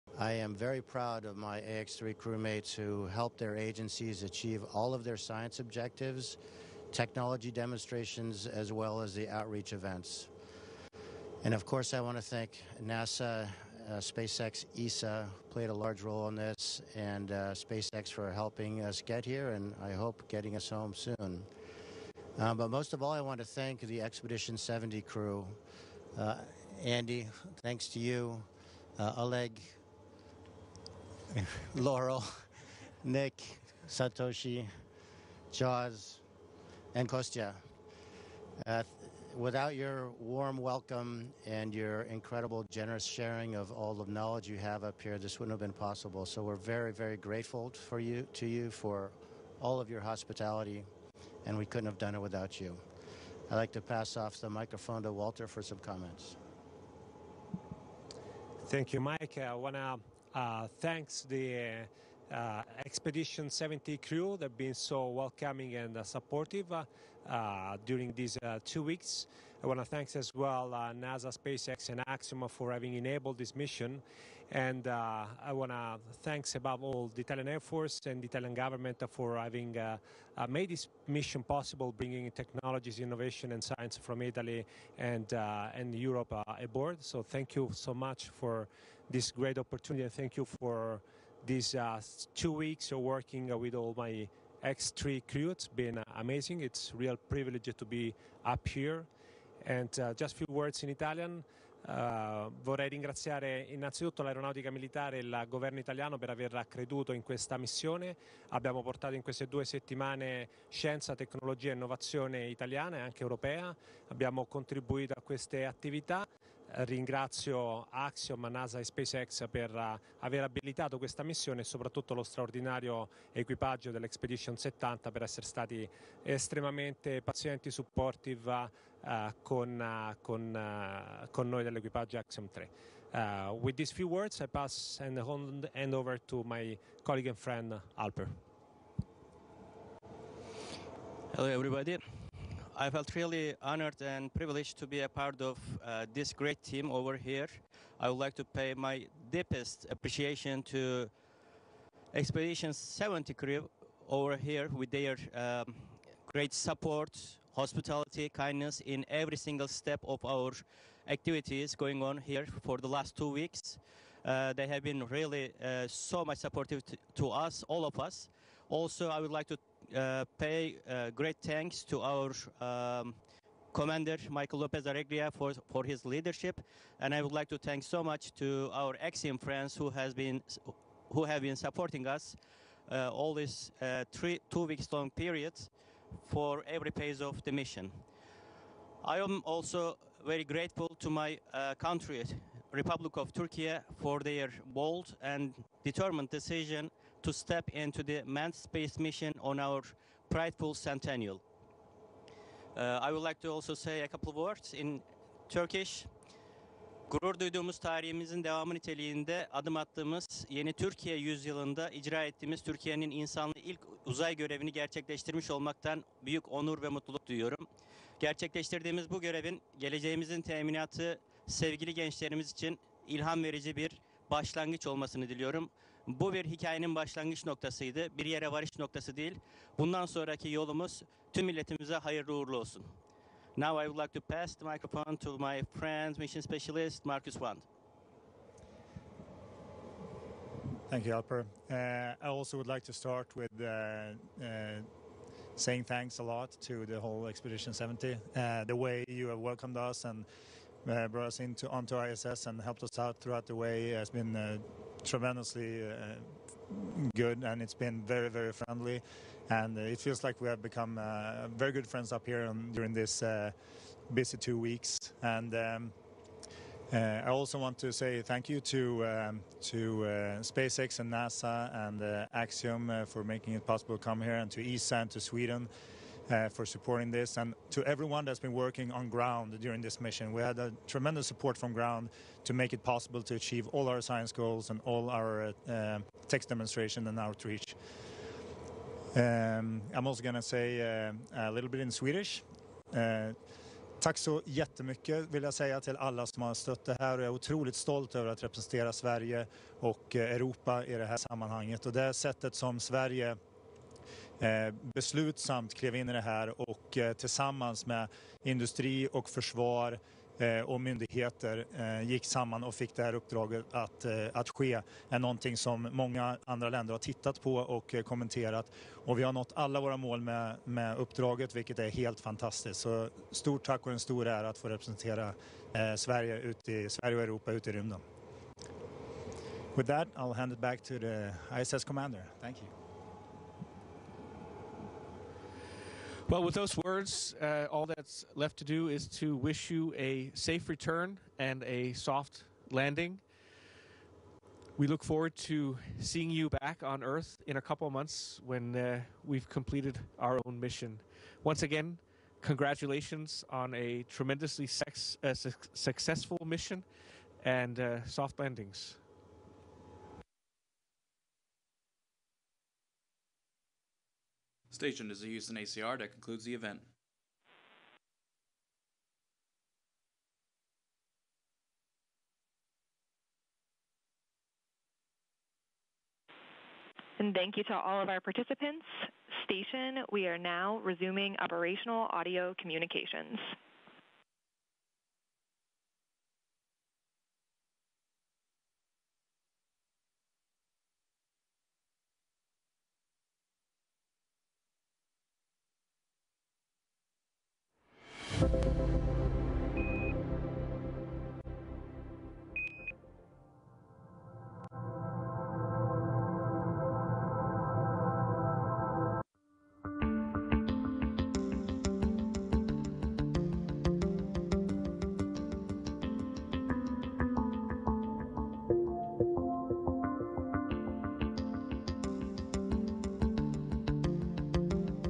LIVE: Axiom Mission 3 crew delivers farewell remarks from ISS
The Axiom Mission 3 crew members deliver farewell remarks from the International Space Station. A SpaceX Crew Dragon capsule carrying the Axiom quartet lifted off atop a Falcon 9 rocket on January 18 from NASA's Kennedy Space Center in Cape Canaveral.